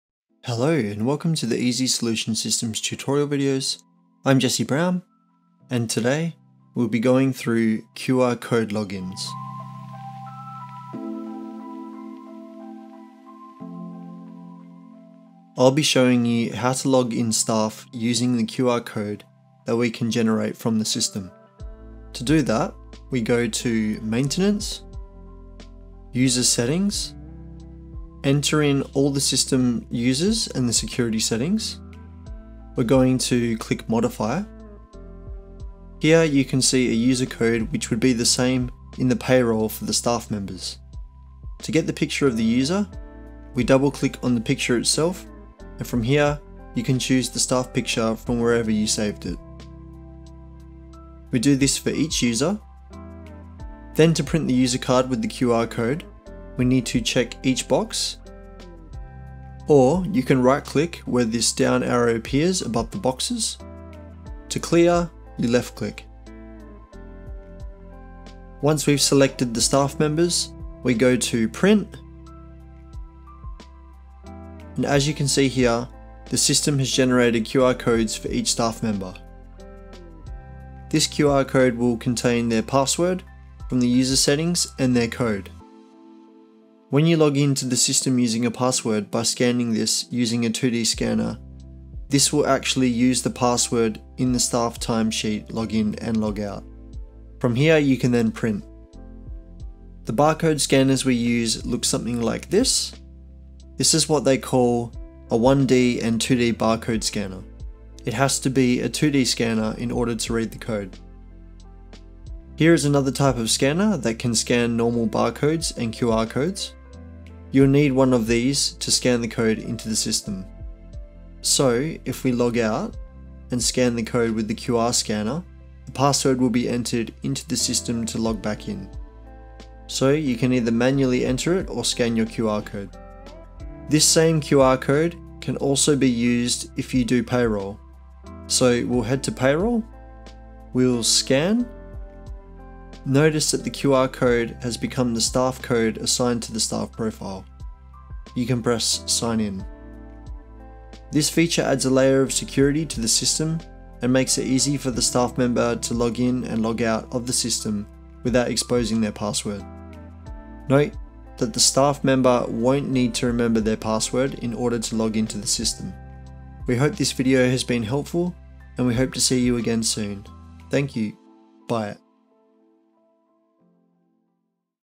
QR code login
Learn how to add, print and use the Staff card with a QR code printed on it to both login to the system or Time Clock login.
This adds both security and convenience to the business and staff.

You can download an obligation free, time limited full version to try for 7 days, and can extend the trial period by 30 days when you email us your company name as it appears in the program.